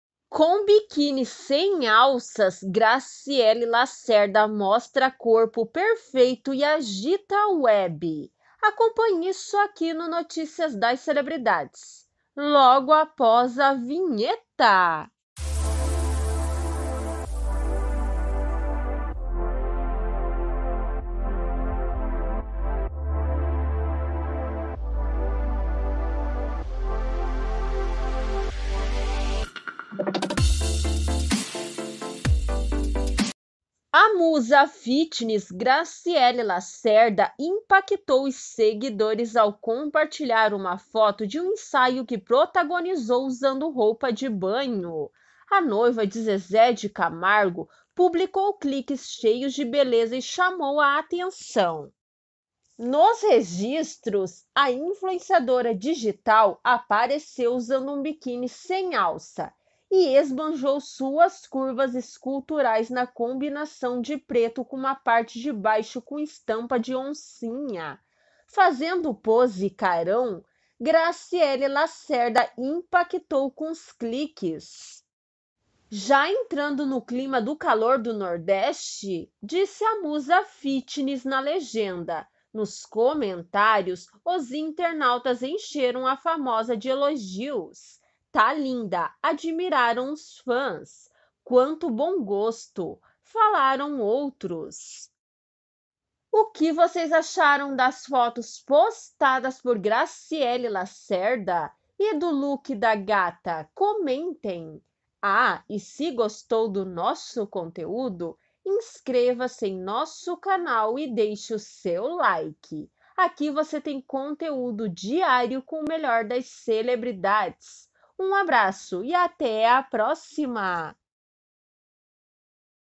COM BIQUÍNI SEM ALÇAS, GRACIELE LACERDA MOSTRA CORPO PERFEITO E AGITA A WEB!
A musa fitness Graciele Lacerda impactou os seguidores ao compartilhar uma foto de um ensaio que protagonizou usando roupa de banho. A noiva de Zezé Di Camargo publicou cliques cheios de beleza e chamou a atenção.

Nos registros, a influenciadora digital apareceu usando um biquíni sem alça e esbanjou suas curvas esculturais na combinação de preto com uma parte de baixo com estampa de oncinha. Fazendo pose e carão, Graciele Lacerda impactou com os cliques.

"Já entrando no clima do calor do nordeste?", disse a musa fitness na legenda. Nos comentários, os internautas encheram a famosa de elogios. "Tá linda!", admiraram os fãs. "Quanto bom gosto", falaram outros.

Assistam o víeo até o final e comentem o que vocês acharam das fotos postadas por Graciele Lacerda! E do look da gata? Comentem!